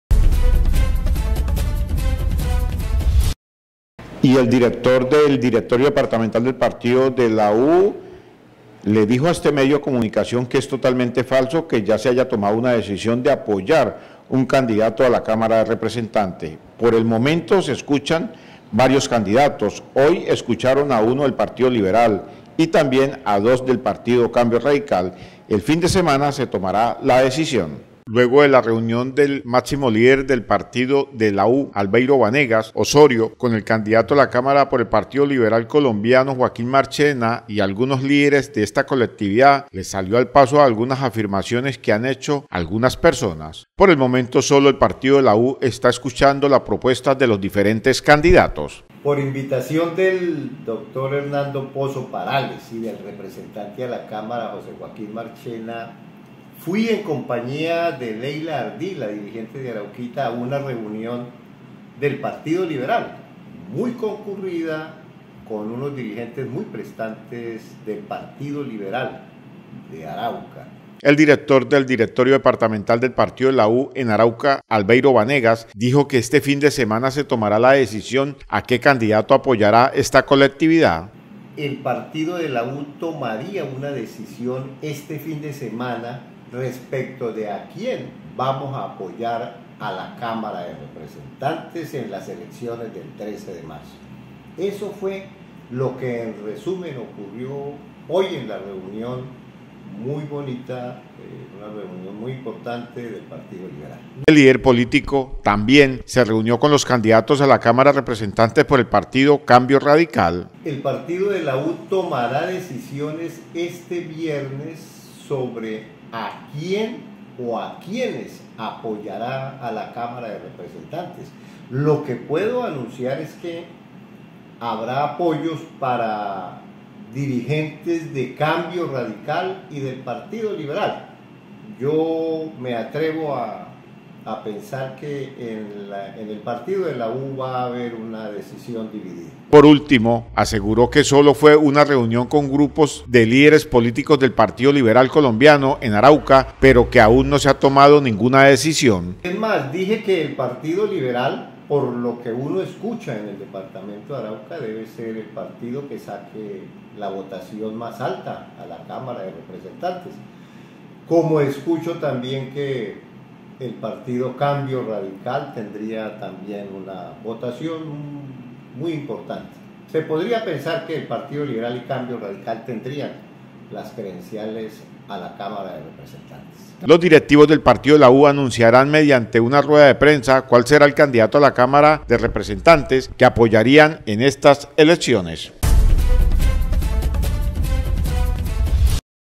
Directivos del Partido de la U en Arauca escucha a los diferentes candidatos a la Cámara
Directivos del Partido de la U en Arauca escucha a los diferentes candidatos, el fin de semana se tomara la decisión a quien apoyarían